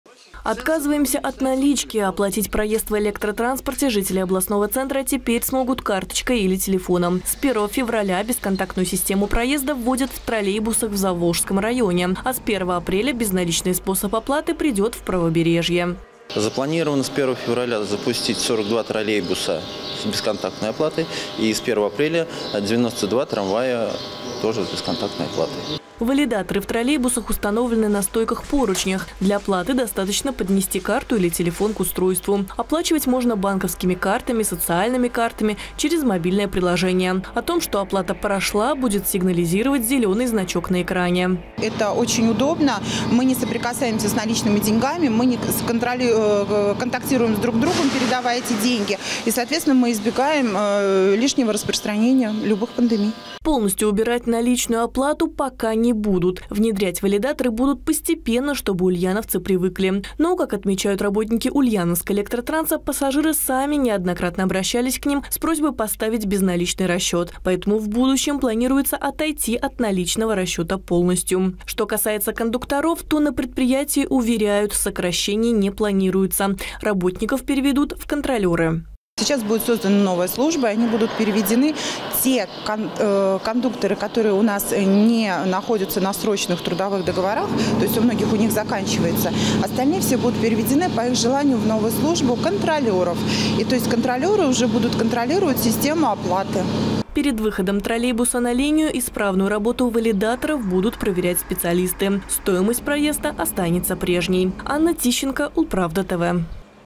Зимой троллейбусы, трамваи – весной. Когда и в каком транспорте Ульяновска введут безналичную оплату
Валидаторы дошли и до ульяновского общественного транспорта. Во многих регионах страны жители успешно оплачивают проезд картой. Ульяновцы не хотят отставать. Первыми безналичный расчет введут в троллейбусах, там уже установили новую систему.